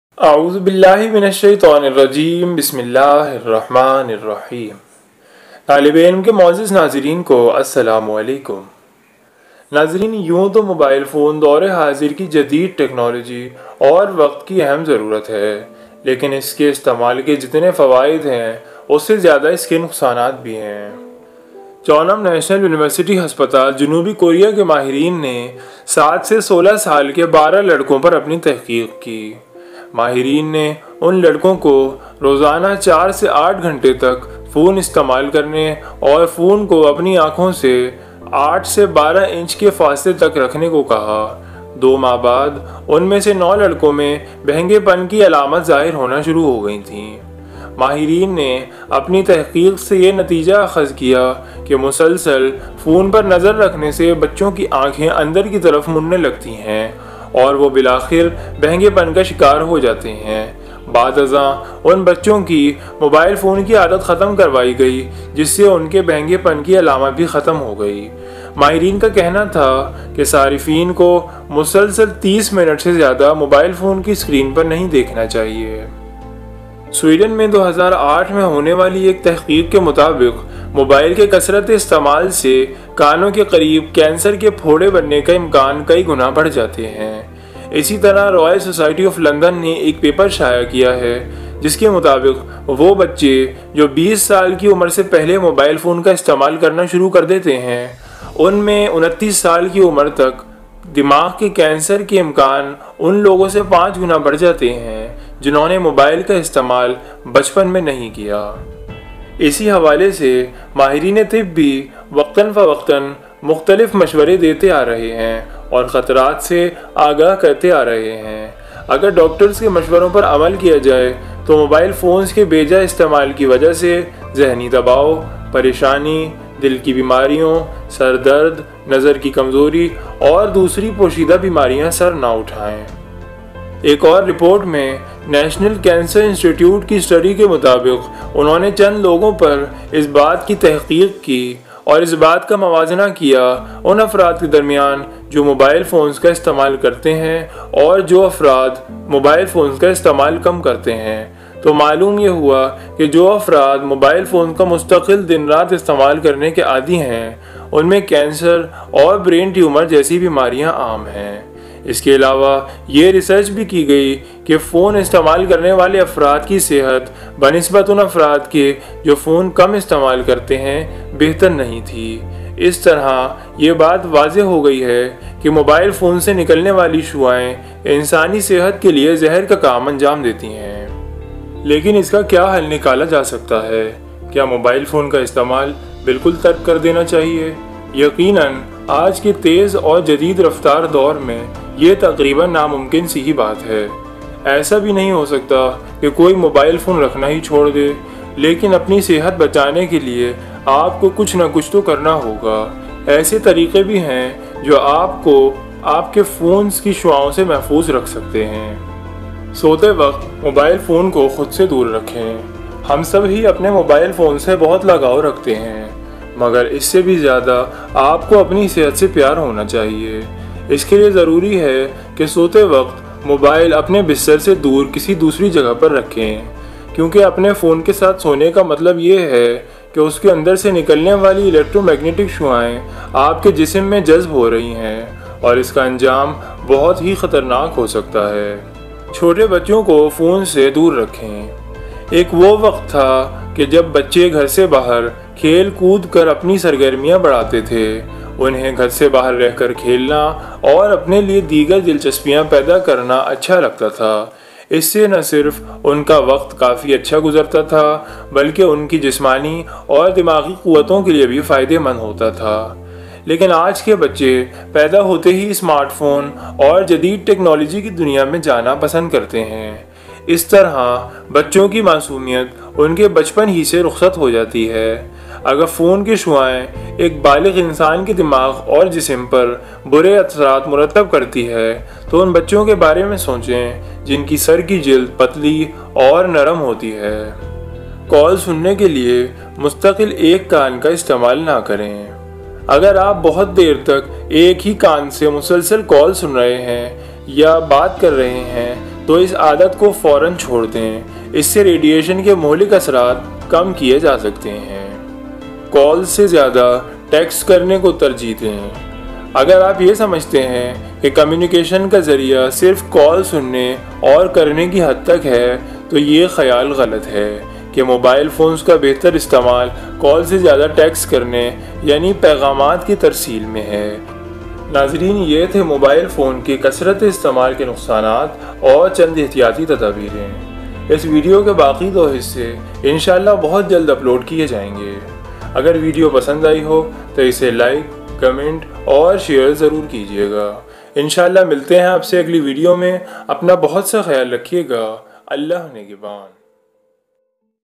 Use of mobile phone and its a harmful effect on human health | Technology | Urdu/Hindi | taLib e iLm
Mobile phone ke istemaal ke khatarnaak nataaij aur sehat per burey asraat | Technology | Urdu/Hindi